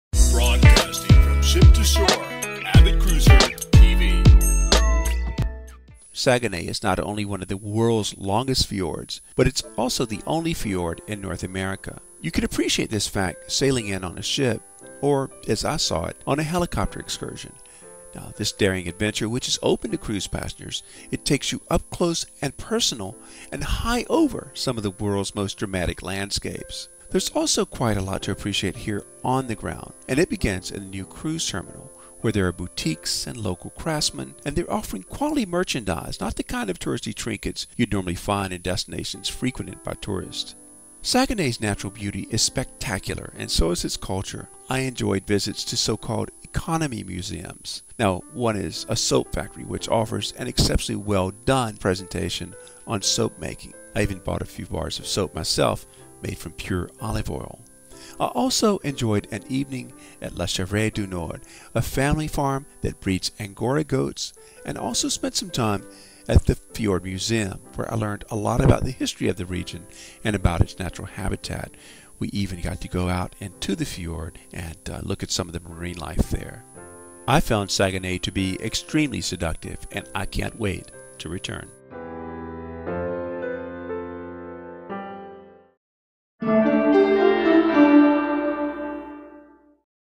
Seductive Saguenay
Saguenay is not only the only fjord in North America but also one of the world's longest fjords.

You can appreciate this fact  sailing in on a ship, or, as I saw it, on a helicopter excursion. The daring adventure takes you up close  to &mdash; and high over &mdash; some of the world's most dramatic landscapes.

There's also quite a lot to appreciate on the ground in Saguenay, and it begins in the new cruise terminal, where there are boutiques and local craftsmen offering quality merchandise, not the kind of touristy trinkets you'd normally find in destinations frequented by tourists.

Saguenay's natural beauty is spectacular, and so is its culture. I enjoyed visits to so-called Economy Museums.

One is the Savonnerie Olivier, which offers an exceptionally well-done and humorous presentation about soap making. I even bought a few bars of the soap, made from pure olive oil.

I also enjoyed an afternoon at Le Chevrier du Nord, a family farm that breeds angora goats.

At the Musee de Fjord, I learned a lot about the history of this region and about its natural habitat. I left Saguenay wanting to return.